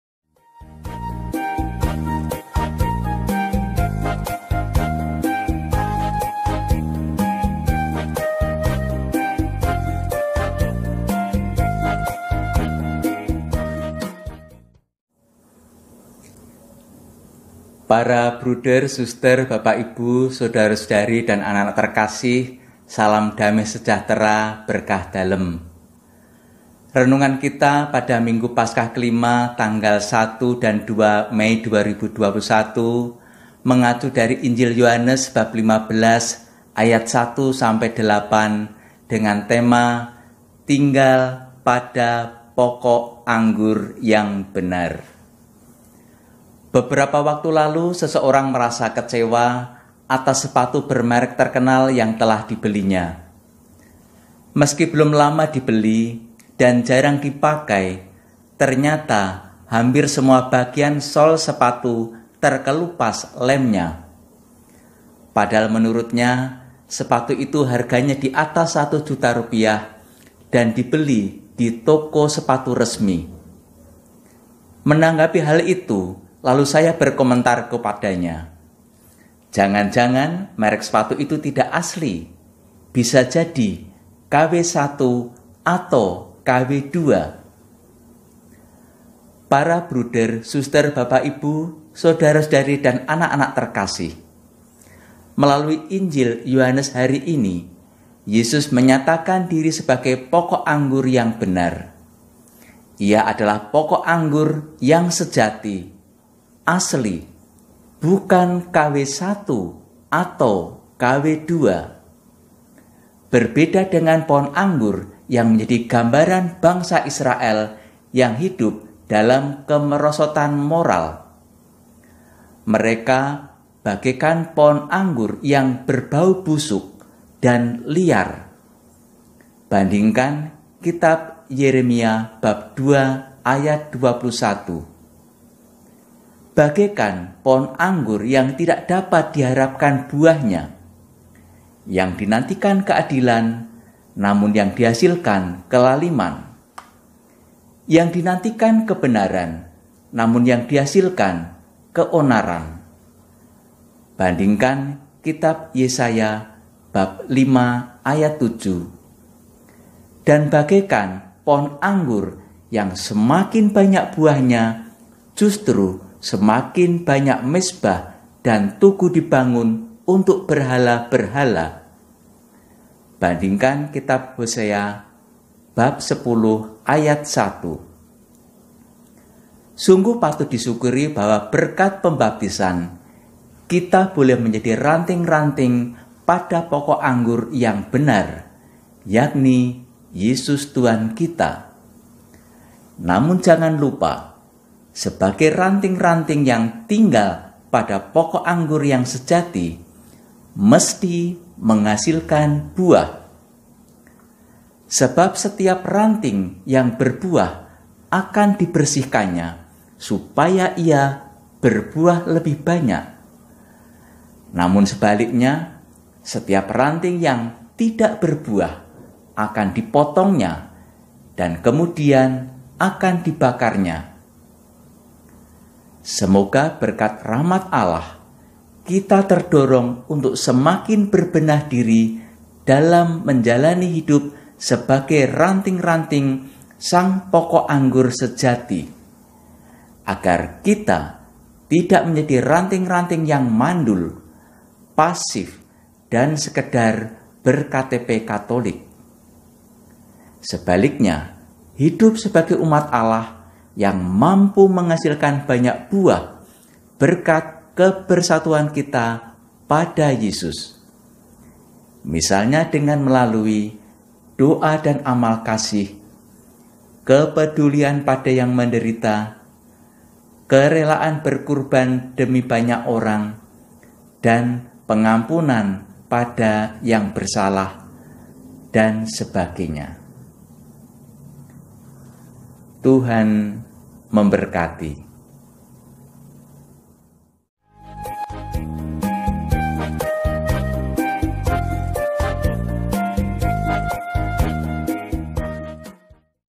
Renungan Minggu Paskah V : Tinggal pada Pokok Anggur yang benar
Suka produk yang ori, atau KW super, KW1, KW2?
Mari kita ikuti apa yang disampaikan romo Aloysius Kriswinarto MSF dalam renungan kali ini.
KW super..atau KW1 ya? Hmmm.